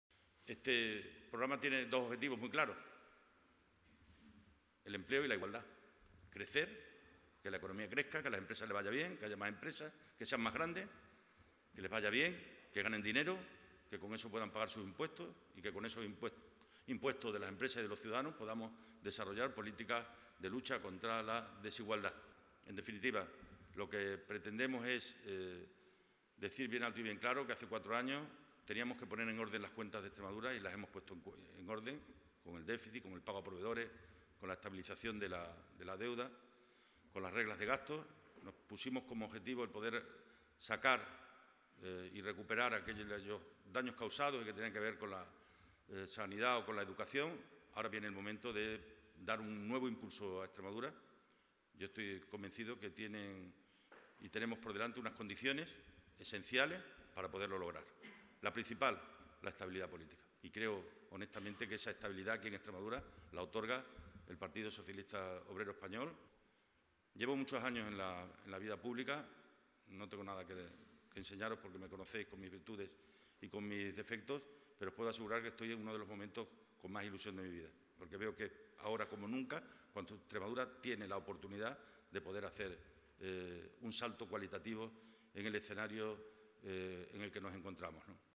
Vara marca empleo e igualdad como "objetivos claros" de su programa
El  secretario general del PSOE de Extremadura, Guillermo Fernández Vara, ha presentado, durante un acto en Mérida, el programa electoral de su partido para los próximos comicios autonómicos; el mismo está compuesto por ocho áreas trasversales y tiene como "objetivos claros" el empleo y la igualdad.